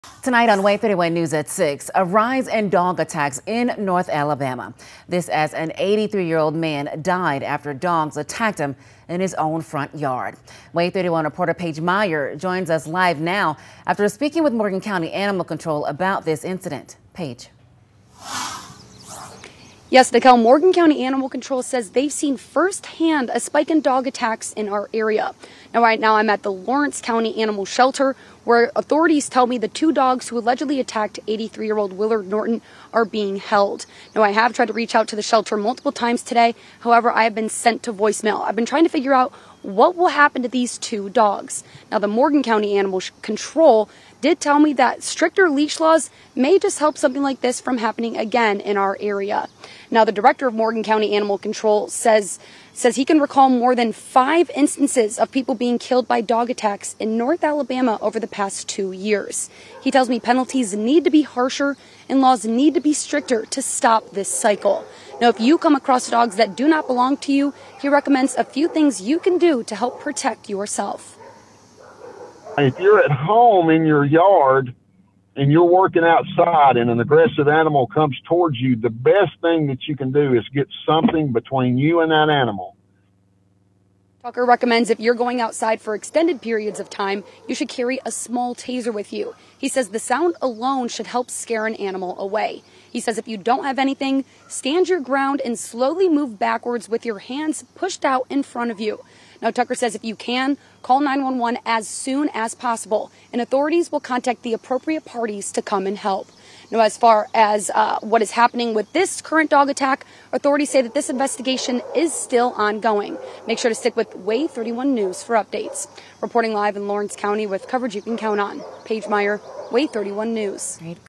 Lawrence County Deadly Dog Attack
Morgan County Animal Control talks about deadly dog attacks.